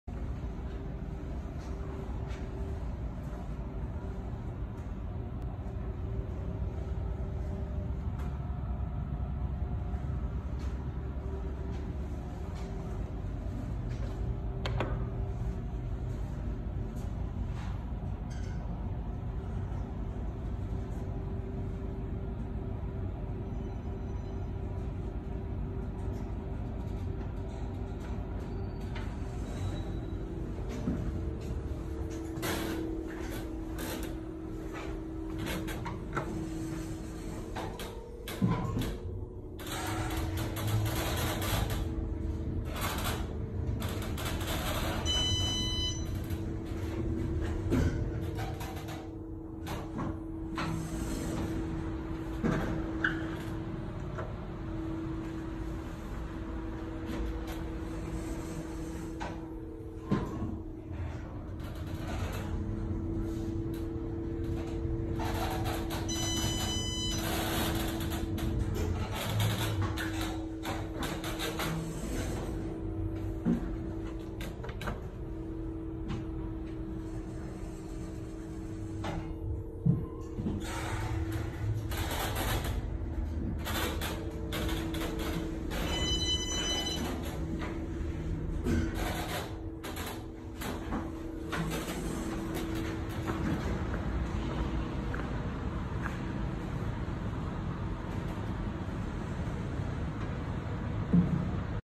Otis Traction Elevator - Crown Center "To Shops" - Kansas City, MO
This is an Otis traction elevator that serves as an entrance "To Shops" in the Crown Center in Kansas City, Missouri.